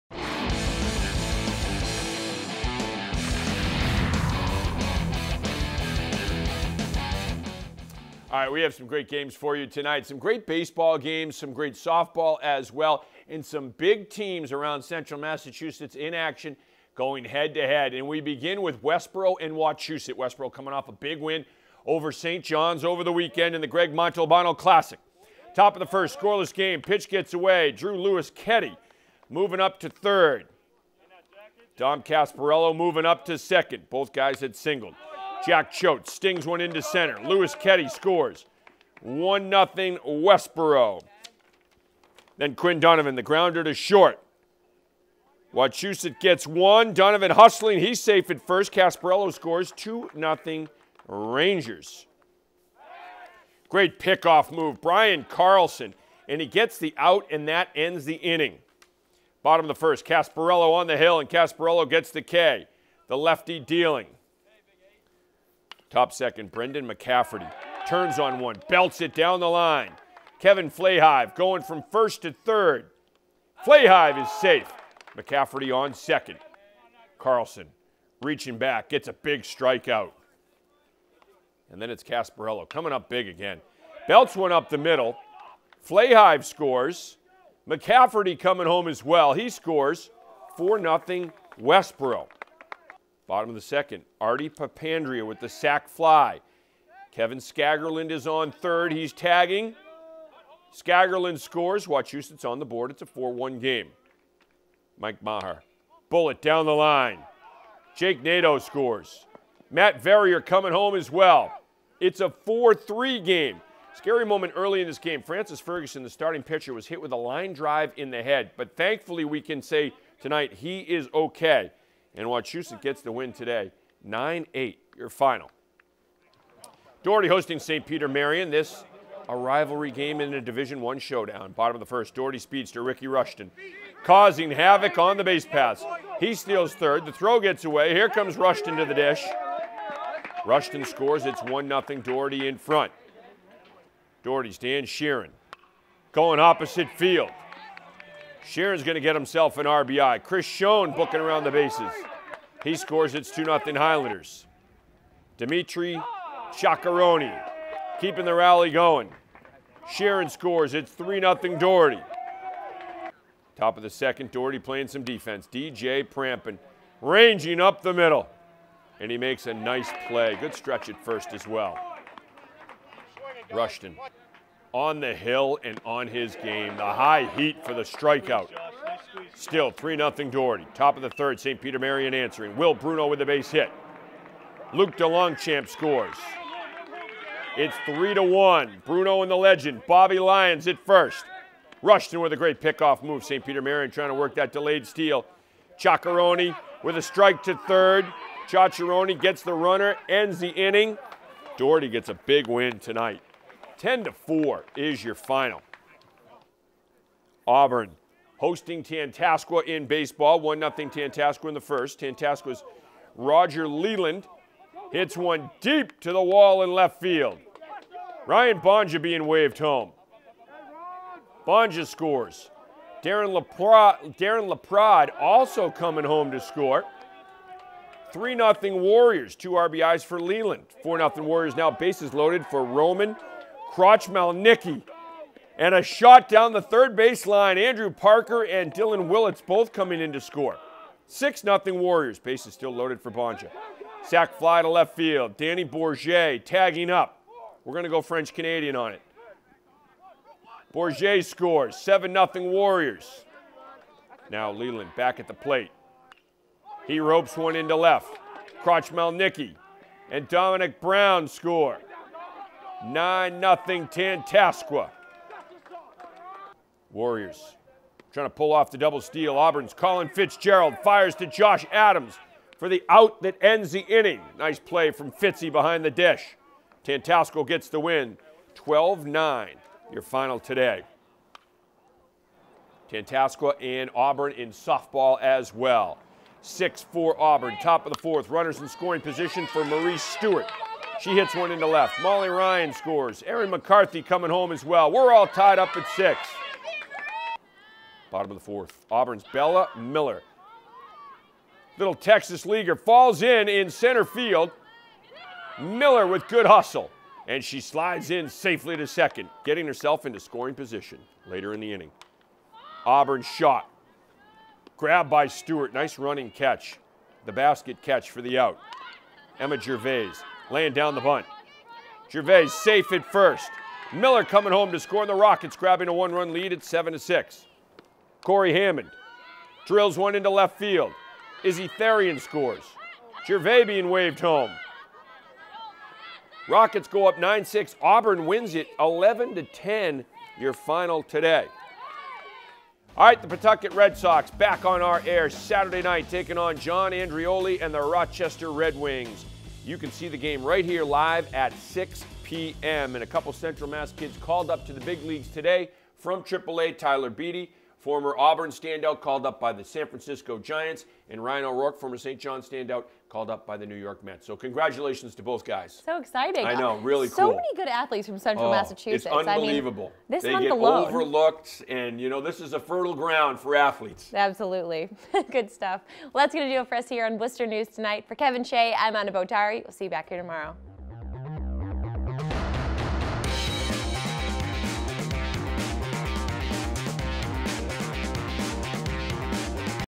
WNT Sports - May 1st, 2019
Westboro-Wachusett, SPM-Doherty, Auburn-Tantasqua Baseball and Tantasqua-Auburn Softball.